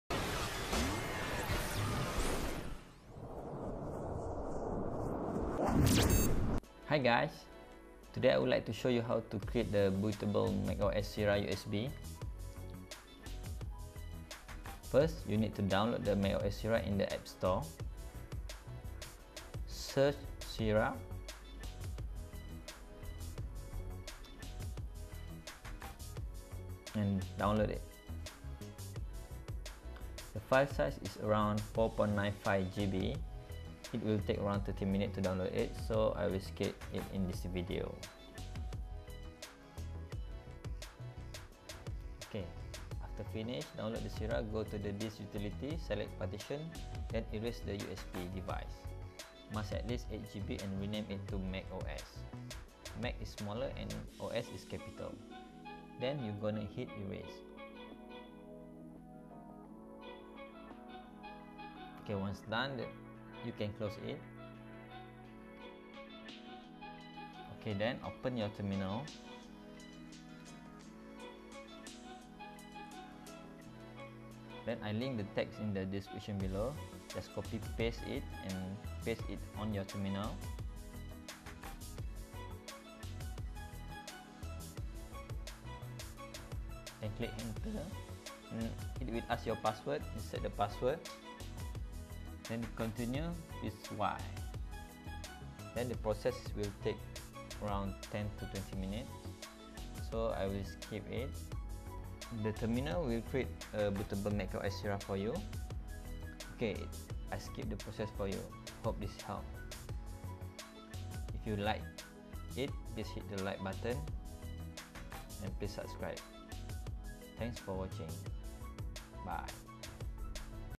How To Create Bootable MacOS Sierra USB
Hey, below is the code for terminal, Your drive MUST be named 'macOS' for it to work.

sudo /Applications/Install\ macOS\ Sierra.app/Contents/Resources/createinstallmedia --volume /Volumes/macOS --applicationpath /Applications/Install\ macOS\ Sierra.app

Enjoy. If you need any help let us know :)